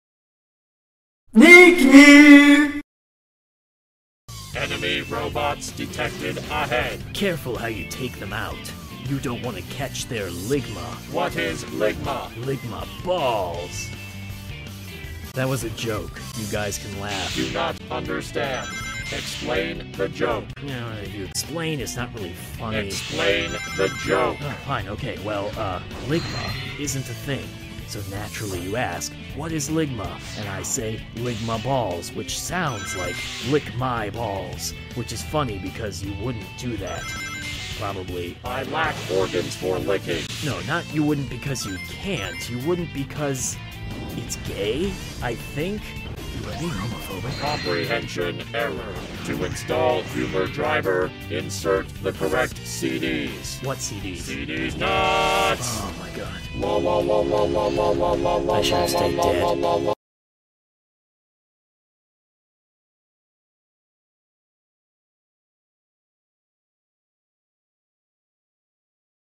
SHADOW THE HEDGEHOG: WHAT IS LIGMA ANIMATED (LEGENDADO)
I saw the @TurboJehtt  video and I decided to animate this!